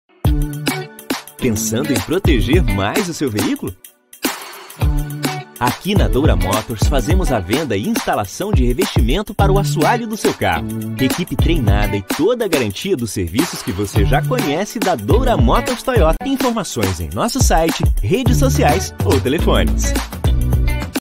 Revestimento de Assoalho é na Douramotors!
Quer proteger e valorizar ainda mais o seu Toyota?

Aqui na Douramotors realizamos a  instalação de revestimento de assoalho, para facilitar a limpeza e proporcionar uma melhor  aparência do seu veículo.

Douramotors, sua concessionária Toyota em Dourados, Três Lagoas e Ponta Porã.